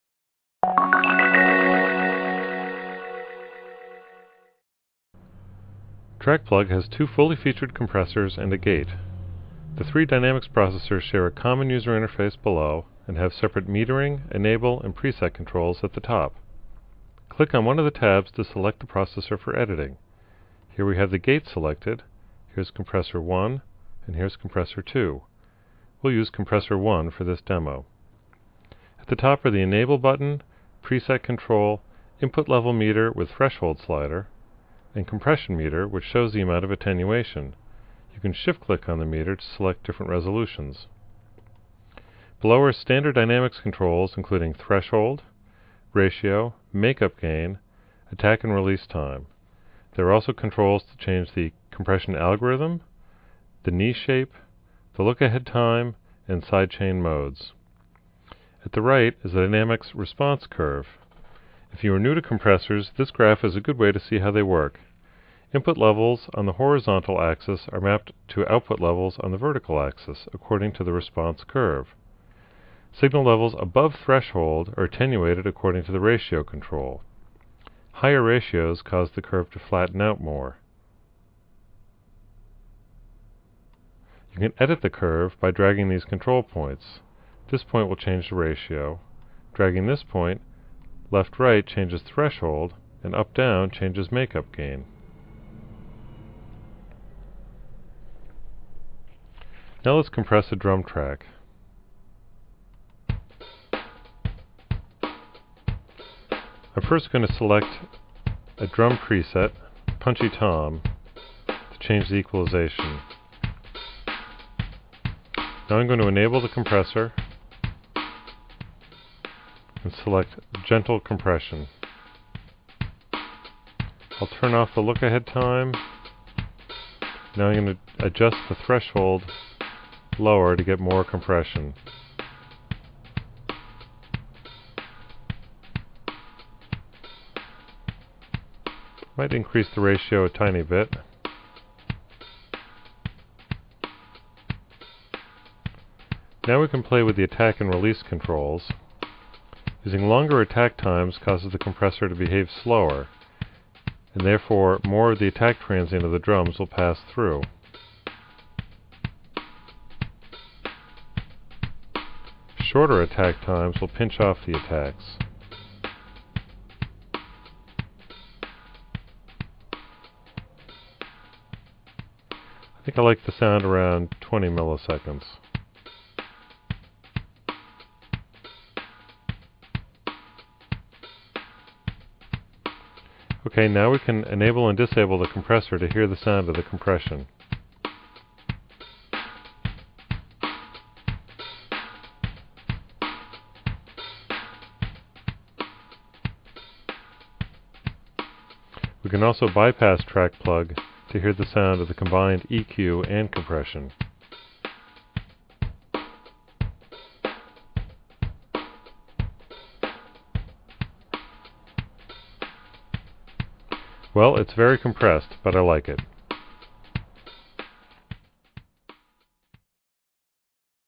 Wave Arts "TrackPlug" Compressor | Tutorial
This is a tutorial video on how to use the "TrackPlug" Compressor to control and shape your audio signal. Wave Arts TrackPlug is the ultimate channel strip with a stunning visual interface design. It has 10 bands of professional 64 bit EQ, dual compressors, a gate, and a peak limiter. Also, its dual EQ comparison sidechain modes allow for precise de-essing. Insert this plug-in for every channel and you wont need anything else in most cases.